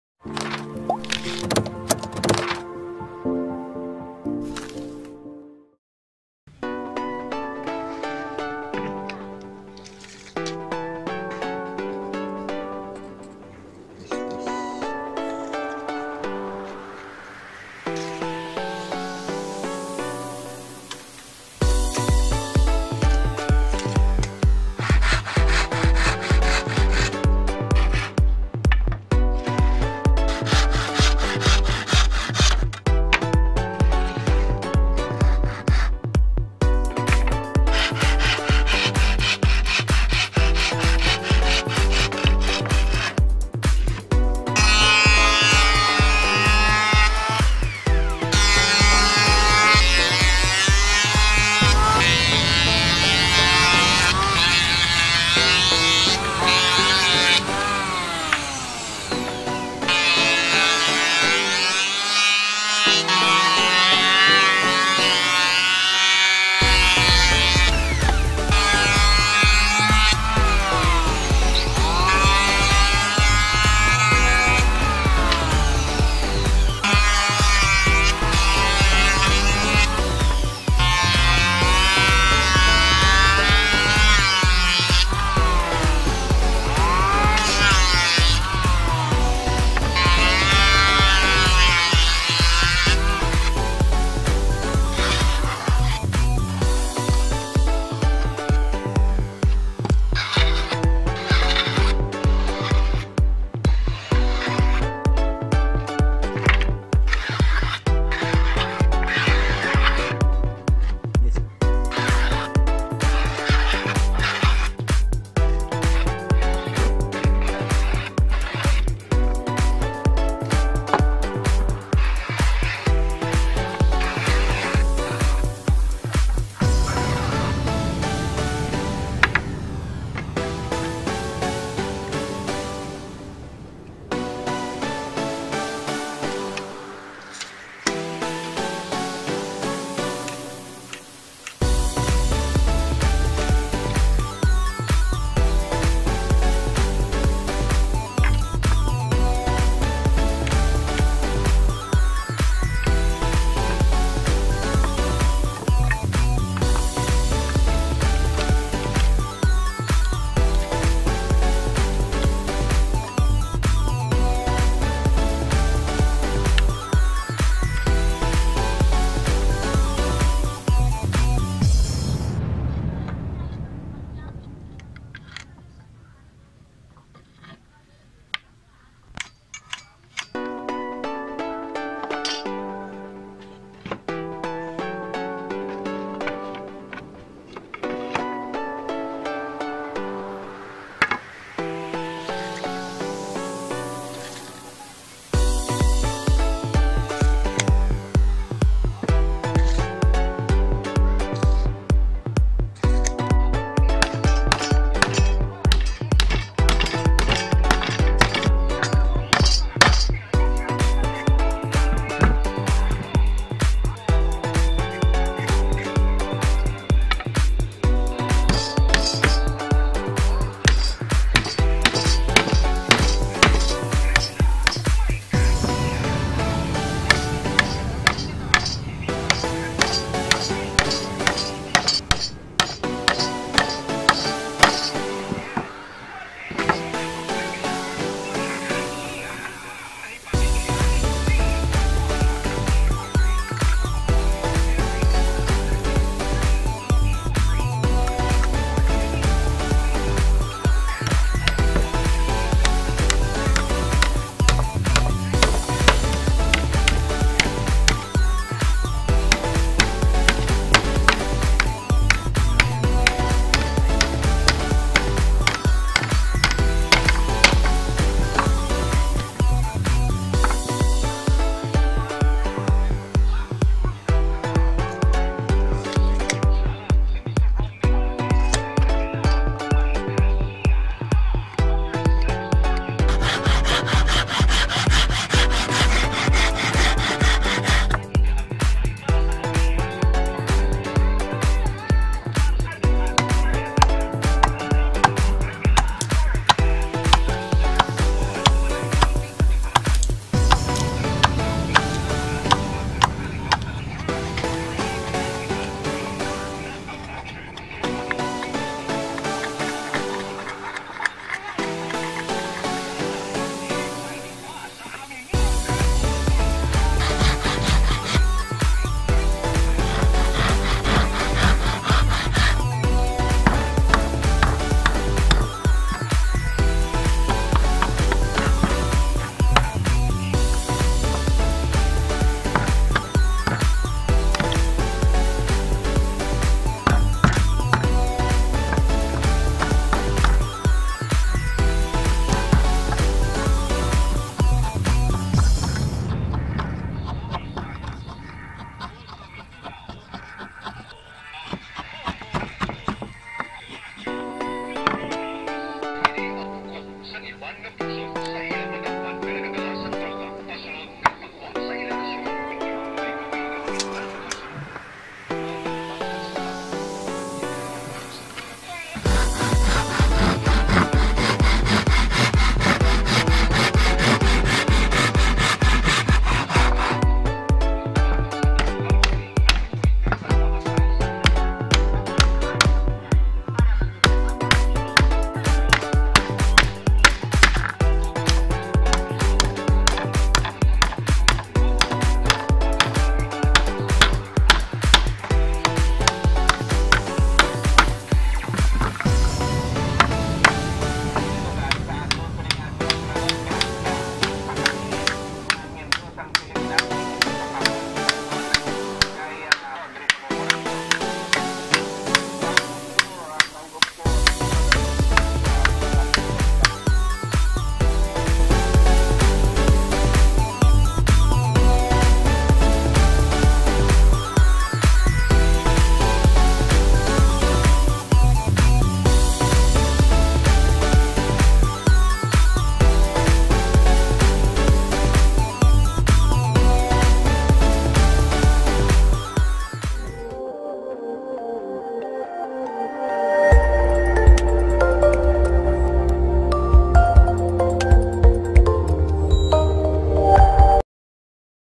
How To Make A Wooden Underarm Crutches
How To Make A Wooden Underarm  Crutches | Woodworking and Furniture Making

Its been a year now since my last upload. But in today's video, I created a wooden underarm crutches. It is built upon from my own ideas and techniques in woodworking.

I hope you enjoy this video and have a nice day everyone.